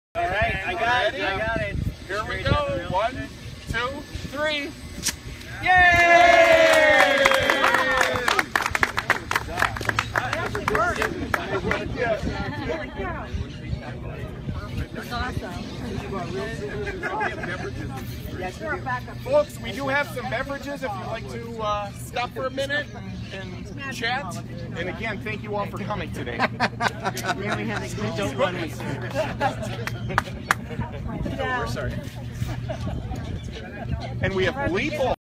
City of Oakland Park, Florida 'Park Lane East' Ribbon Cutting ceremony
bleeple covered the ribbon cutting of Park Lane East in Oakland Park, Florida.